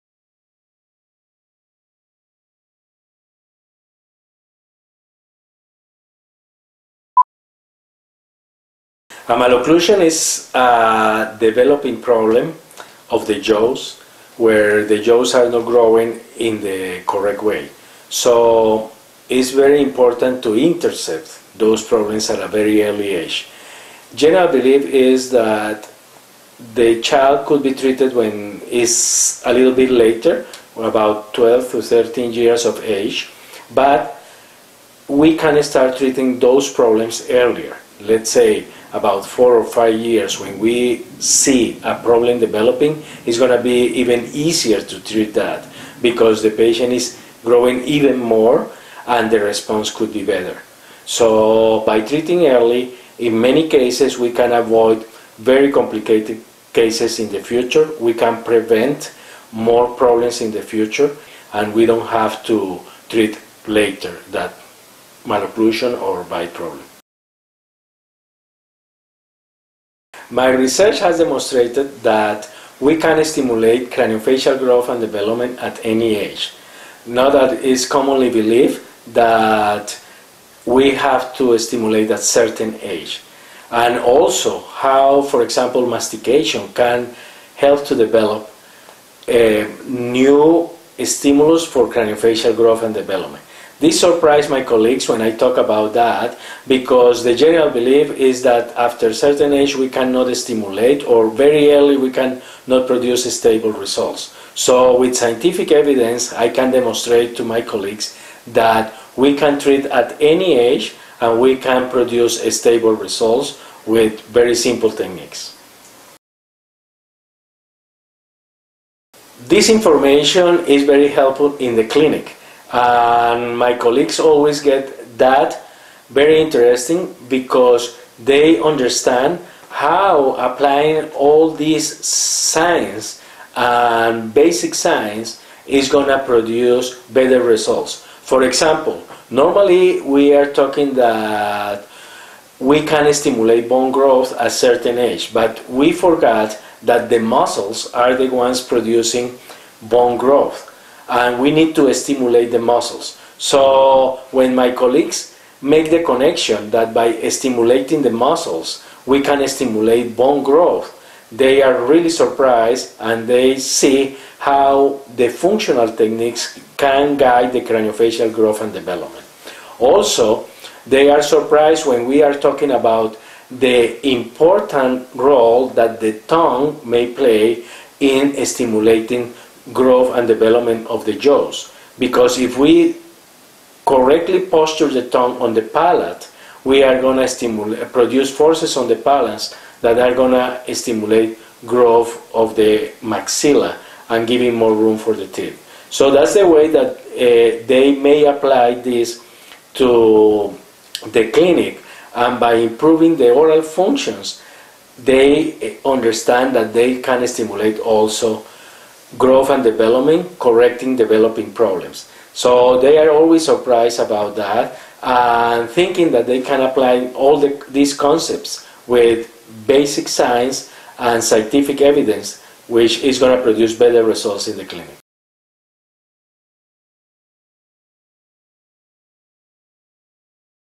Dr. German Ramirez Interview
1. What is a Malocclusion
2. Dr. Ramirez Research
3. Helpful teaching in the clinic